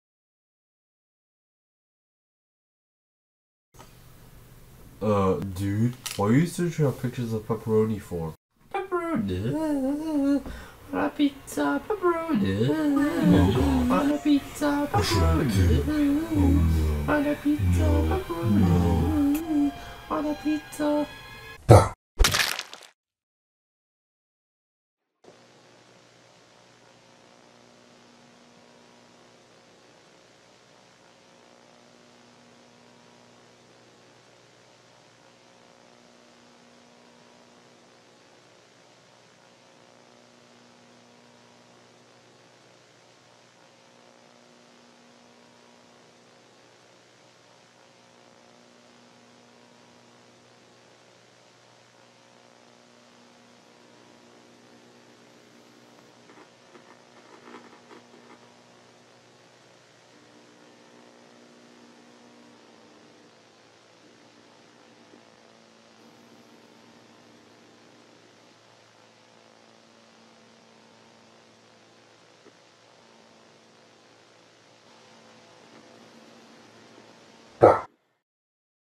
The Pepperoni Incident
We are still finding out how Oswald's roommate got possessed by the Phantom Pepperoni